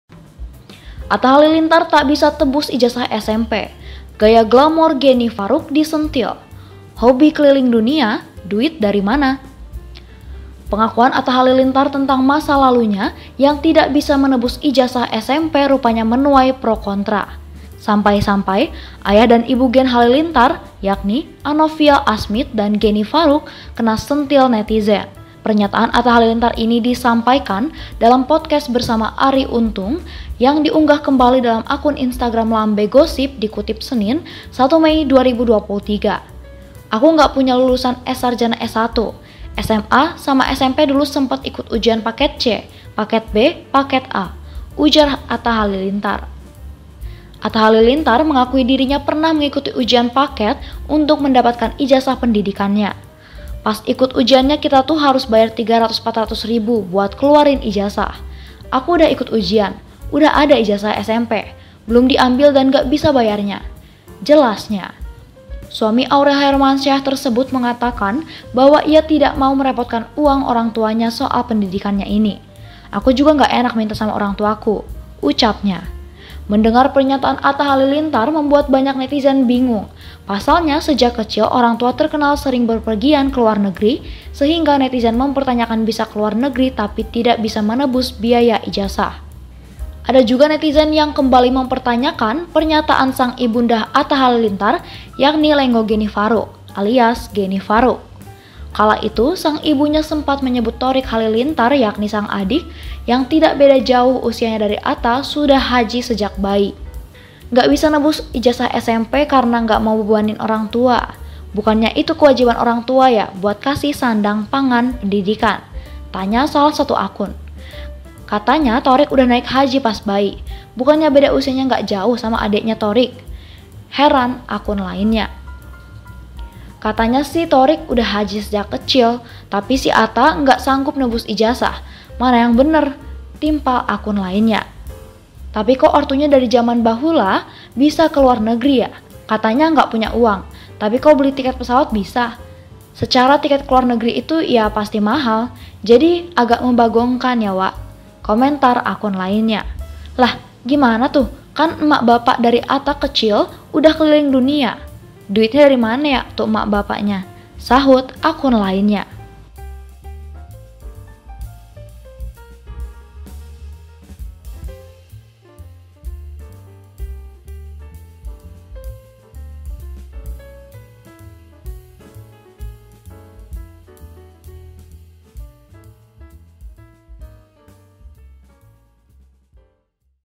Atta Halilintar Tak Bisa Tebus Ijazah SMP, Gaya Glamor Geni Faruk Disentil: Hobi Keliling Dunia Duit
Atta Halilintar Tak Bisa Tebus Ijazah SMP, Gaya Glamor Geni Faruk Disentil: Hobi Keliling Dunia Duit Dari Mana?

Pengakuan Atta Halilintar tentang masa lalunya yang tidak bisa menebus ijazah SMP rupanya menuai pro kontra. Sampai-sampai, ayah dan ibu Gen Halilintar, yakni Anofial Asmid dan Geni Faruk kena sentil netizen.

Pernyataan Atta Halilintar ini disampaikan dalam podcast bersama Arie Untung yang diunggah kembali dalam akun Instagram lambegosiip, dikutip Senin (1/5/2023). "Aku nggak punya lululsan S Sarjana S1. SMA sama SMP dulu sempet ikut ujian paket c, paket b, paket a," ujar Atta Halilintar.